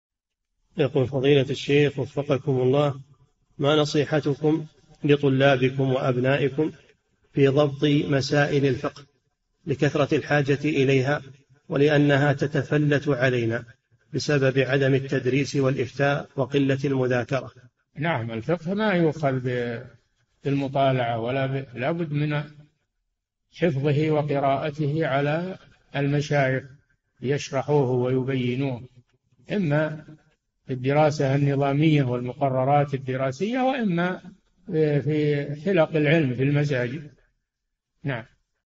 [3086- 4717] كيفية ضبط مسائل الفقه؟ - الشيخ صالح الفوزان
[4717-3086] ما نصيحتكم لطلابكم وأبنائكم في ضبط مسائل الفقه لكثرة الحاجة إليها ولأنها تتفلت علينا بسبب عدم التدريس والإفتاء وقلة المذاكرة؟
الجَوَابُ: نعم الفقه ما يؤخذ بالمطالعة لا بُدّ من حفظه وقراءته على المشايخ ليشرحوه ويبينوه، إمّا بالدراسة النظامية والمقررات الدراسية وإمّا في حلق العلم بالمساجد.
السيرة الذاتية :
للاشتراك في قناة الشيخ صالح الفوزان
لمشاهدة حلقات فتاوى على الهواء كاملة

للاستفسار والإقتراحات والدعم
رئيس الفريق
لمشروع كبار العلماء